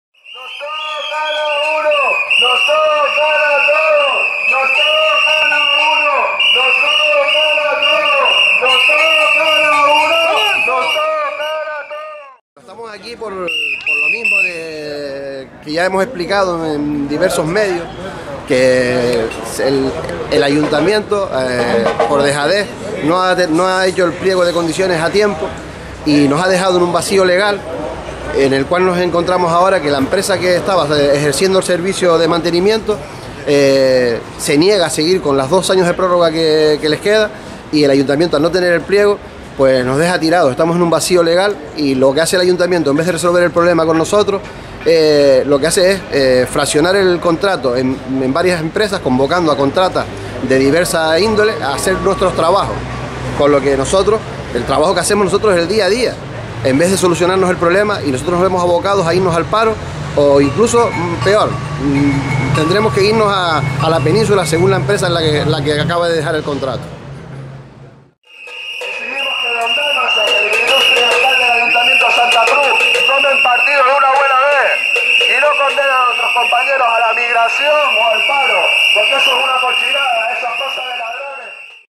Protesta de trabajadores/as de Elecnor en Ayuntamiento de Santa Cruz
Desde Sí se puede apoyamos las reivindicaciones y nos solidarizamos con el colectivo de trabajadores y trabajadoras del servicio de mantenimiento, reforma, ampliación y mejora de los colegios públicos, dependencias y edificaciones municipales.